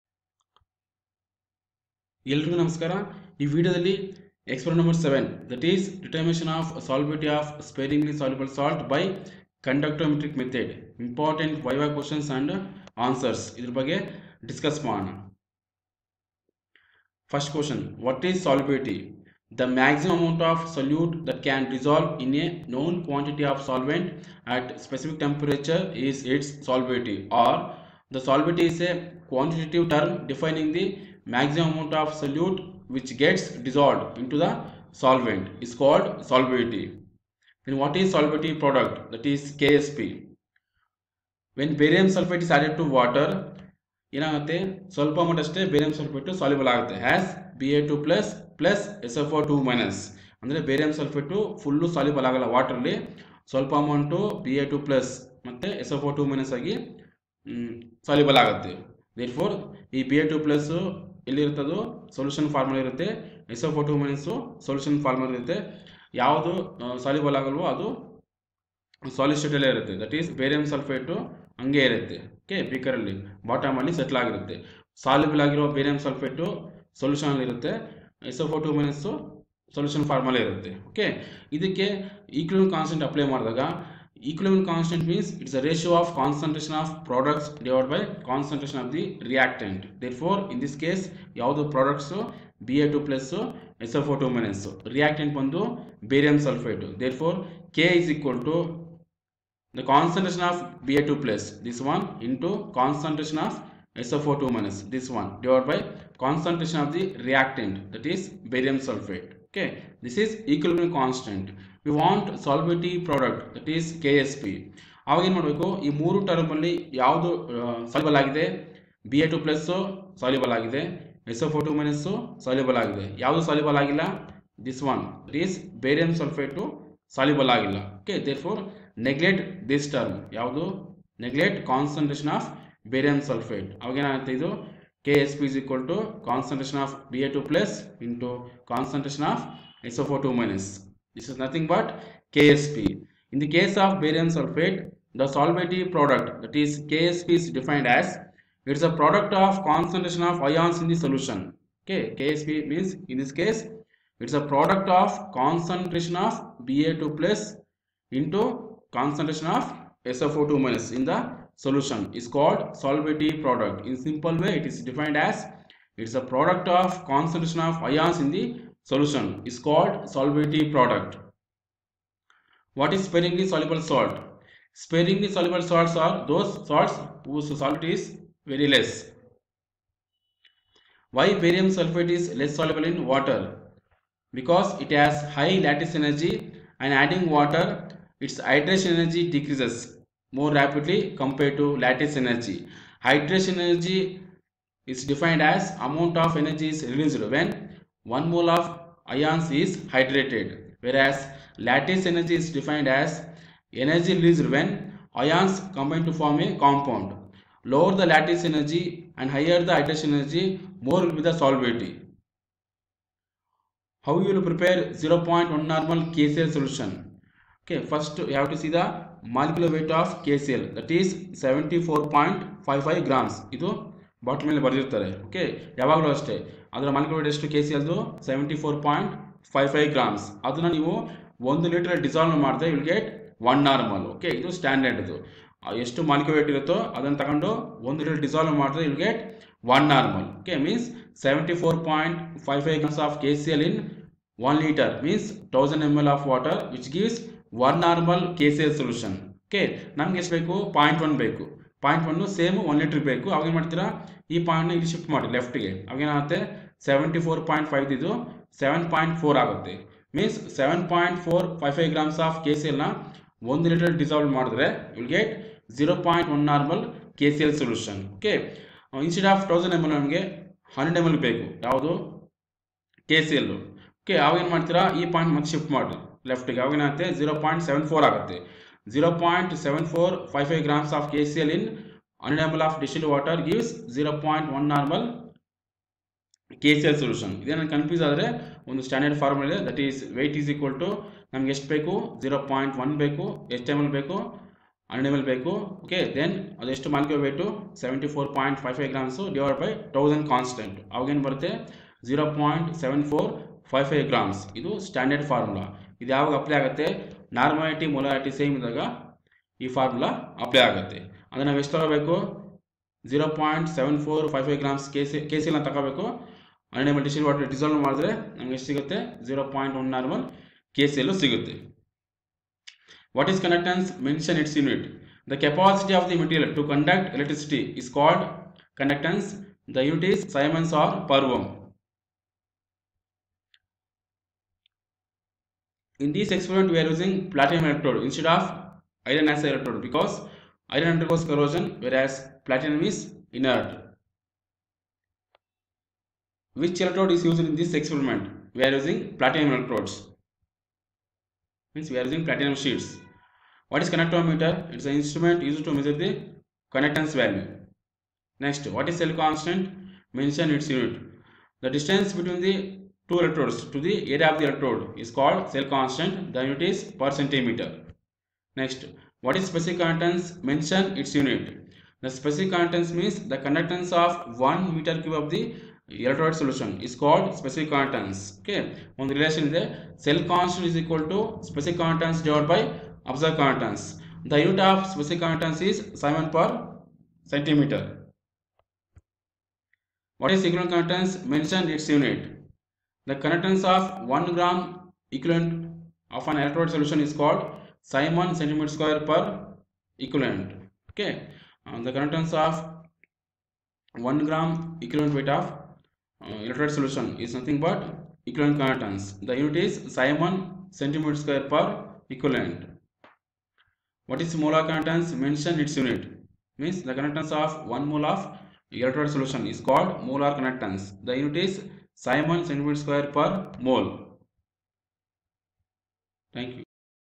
Experiment 7 Viva Determination of solubility of sparinglysoluble salt (BaSO4) by conductometrically
Experiment 7: Determination of solubility of sparingly soluble salt (BaSO4) by conductometric method